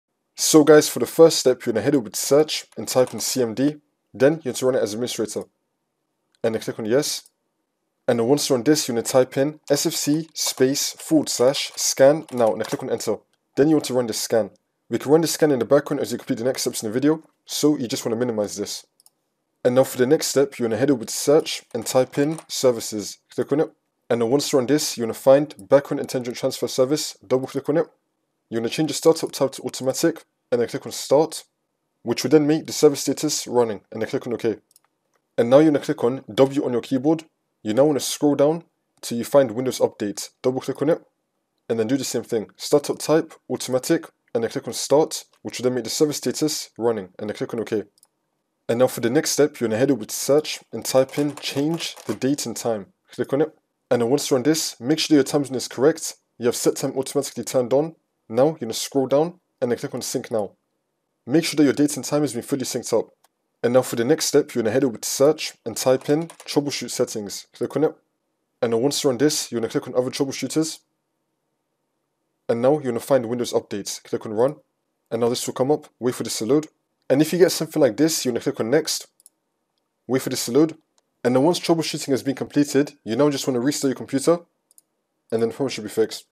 How To Fix Error Codes 0xc1900101 0x30018 0x20017 OR STALLS AT 40% in Windows
How To Fix Error Codes 0xc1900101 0x30018 0x20017 in Windows

In this video, I'll show you how to fix the Windows error codes 0xc1900101, 0x30018, and 0x20017. These errors often occur during Windows updates or installations, and I'll guide you through the steps to resolve them and get your system working properly again.

This guide provides steps to resolve the Windows 10 or windows 11 upgrade error 0xC1900101 – 0x4001E, which states: "The installation failed in the SECOND_BOOT phase during PRE_OOBE operation."

Details about this issue: When upgrading to a newer Windows 10 build or transitioning from Windows 7/8.1 to Windows 10 or 11, the process may fail with the error mentioned above and revert to the previous version.